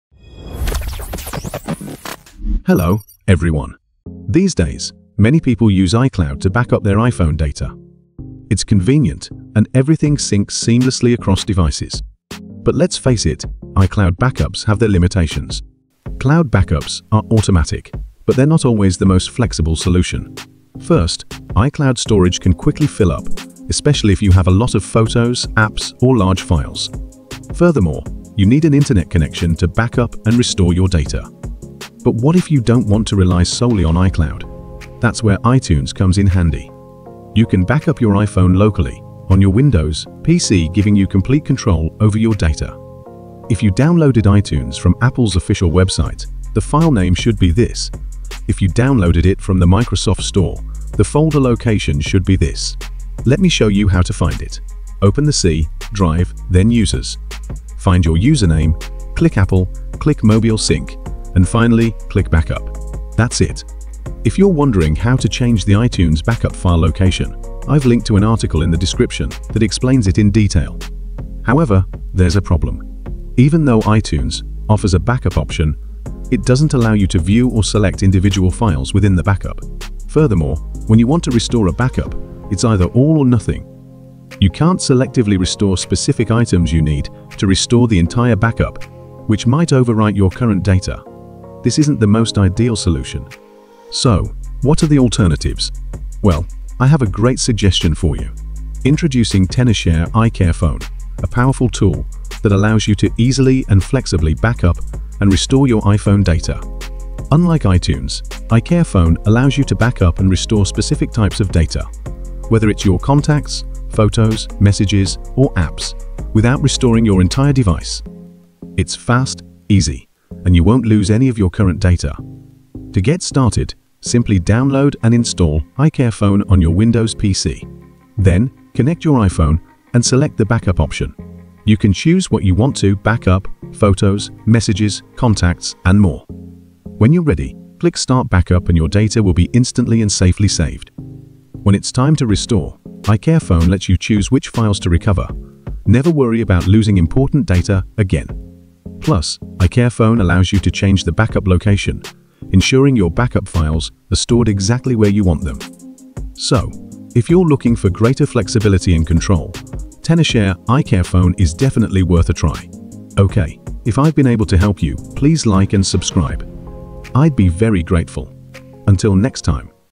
How to Change iTunes Backup Location in Windows 10/11 - Find iTunes Backup Location!
How to Change iTunes Backup Location on Windows 10/11 and Mac [Sept 2025 Updated]：

Want to know how to find and change your iTunes backup location on Windows 10/11? In this video, we’ll walk through where iTunes stores your backups and explain the limitations of iTunes backups. You’ll see why restoring from iTunes can be frustrating—and why a smarter alternative exists.With Tenorshare iCareFone, you can back up and restore iPhone data more flexibly: selectively choose files, avoid overwriting current data, and even change your backup location with ease.

How to Transfer Data From Old iPhone to iPhone 17 Without iTunes

⏱ TIMESTAMP:
00:00 Intro
00:21 Limitations of iCloud
00:35 Using iTunes
00:35 How to Find iTunes Backup Location
01:38 Backup with Tenorshare iCareFone

📌 Condensed Video Bullet Points
Intro: Most people use iCloud for iPhone backups → convenient but limited (storage fills quickly, internet required).

iTunes Backup: Local backup on Windows PC → more control, but files are hidden and unreadable.

How to find backup location:
·C: → Users → [Your Username] → Apple → MobileSync → Backup.
·Location differs depending on whether iTunes is from Apple’s website or Microsoft Store.
·Limitation: iTunes restores everything at once → can’t select files → may overwrite current data.

Solution: Tenorshare iCareFone.
·Selective backup & restore (photos, messages, contacts, etc.).
·No risk of overwriting existing data.
·Option to change backup location easily.

How to use iCareFone:
·Download & install → connect iPhone → choose “Backup” → select data type → click “Start Backup.”
·Restore only the files you need.